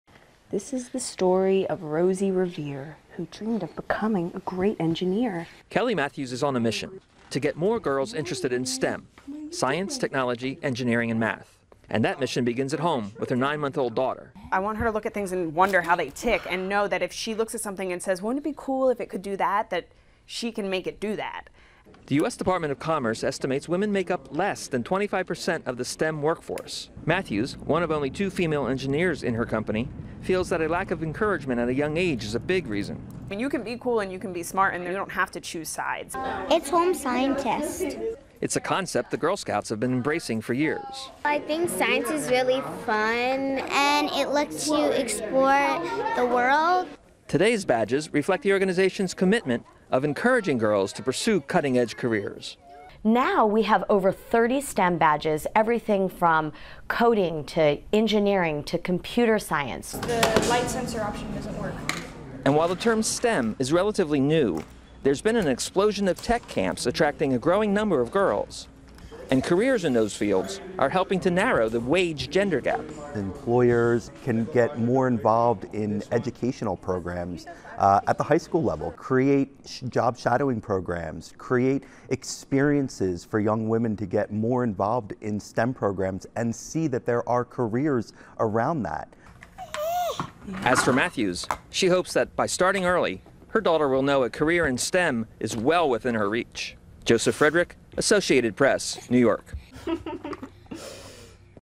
Creating Lifelong Love of Science and Math
Kelly Mathews is a new mom on a mission to get girls interested in science, technology, engineering and math, and it starts with her own daughter. The Girl Scouts are doing their part, too, by promoting S.T.E.M. through badges and activities. (Nov. 18)